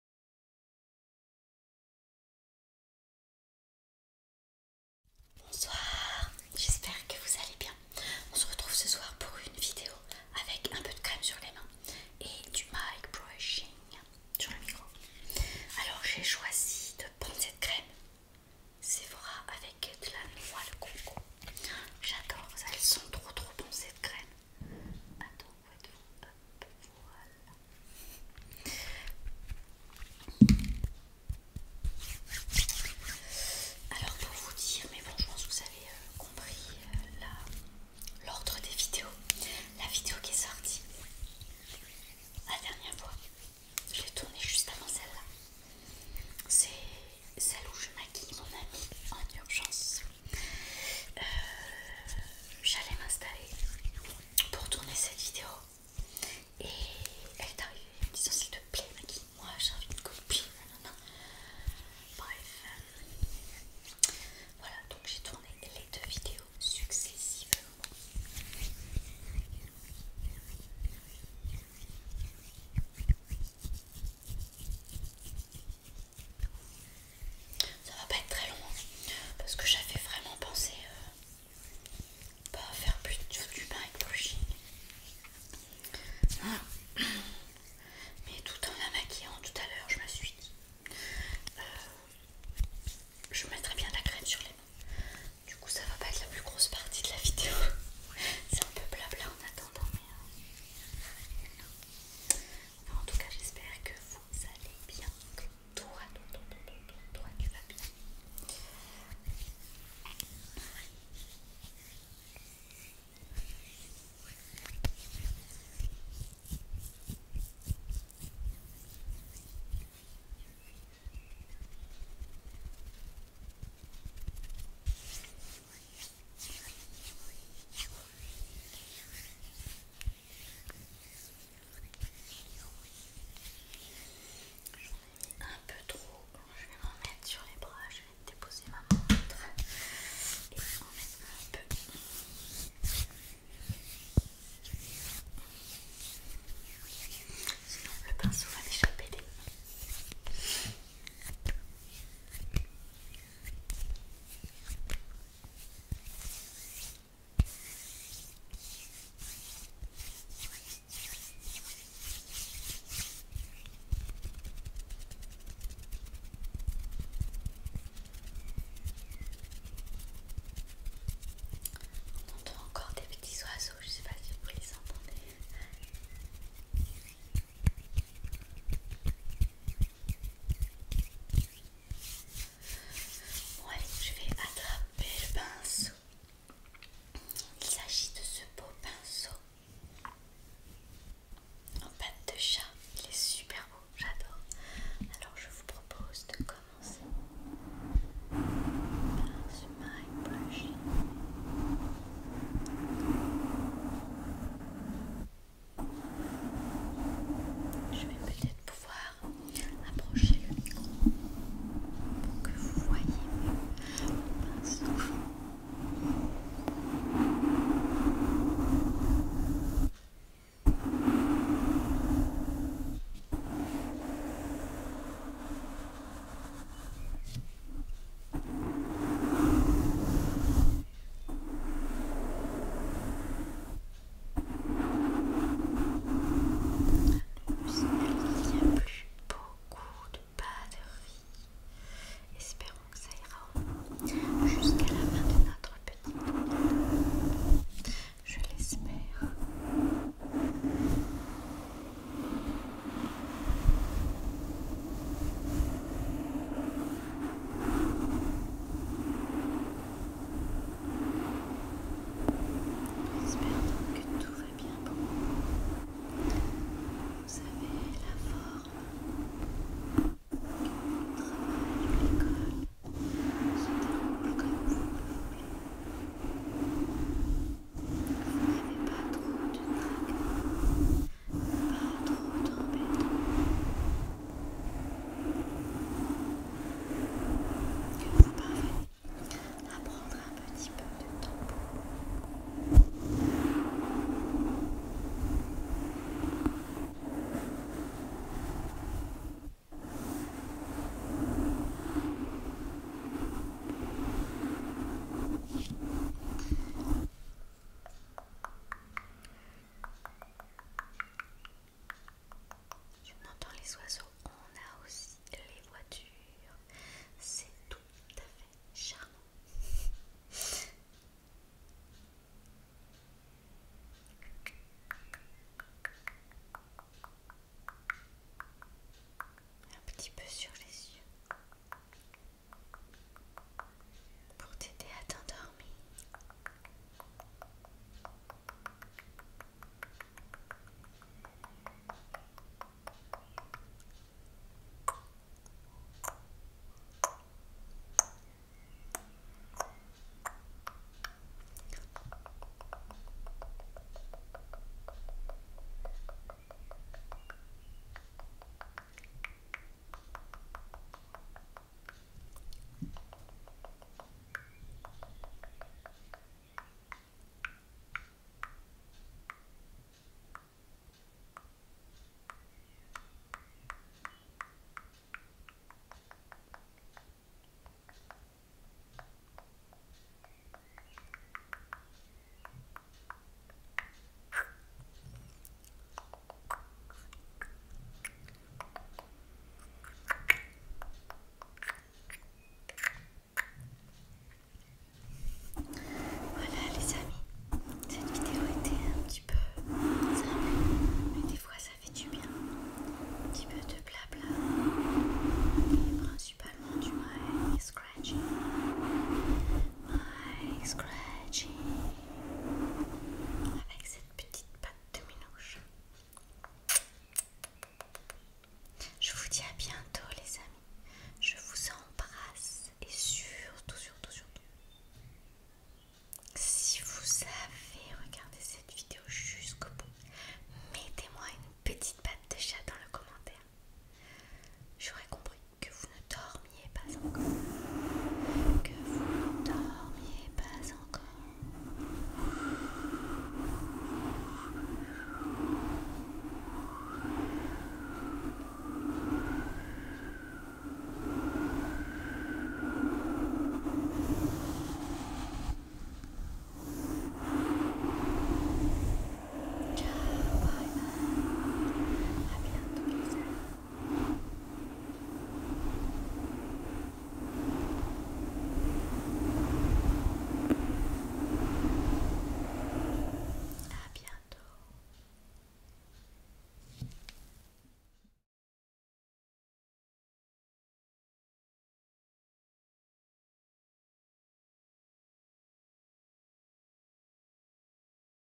ASMR Français - Crème sur les mains et pinceau "patte de chat"
Bienvenue chez moi !

Voici la vidéo prévu initialement vendredi, mais qui a été interrompue par mon amie qui est passée se faire maquiller !!

N’hésitez pas à commenter ça m’aide énormément pour le référencement et merci de vos petits pouces 👍🏻

Bizoux 😘
LN 🌺